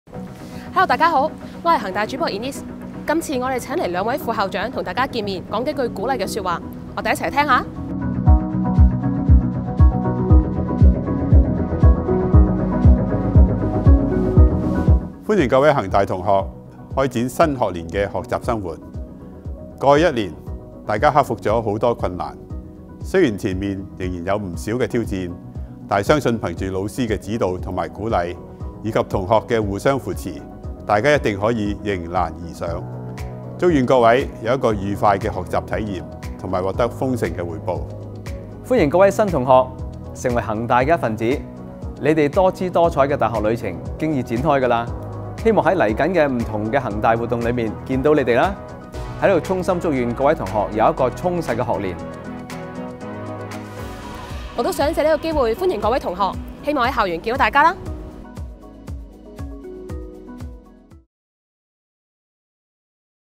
【恒大人 與你同行】開學啦！副校長篇
今次 恒大主播 Inez邀請左恒大副校長 (學術及研究) 許溢宏教授及副校長 (機構發展) 方永豪博士，喺開學前同大家講幾句勉勵嘅說話，一齊黎睇下啦!

This time, our HSUHK Host Inez has invited Professor Y V Hui, Vice-President (Academic and Research), and Dr Tom Fong, Vice-President (Organisational Development) to share a few words of encouragement before the new academic year starts. Let's check out the video now!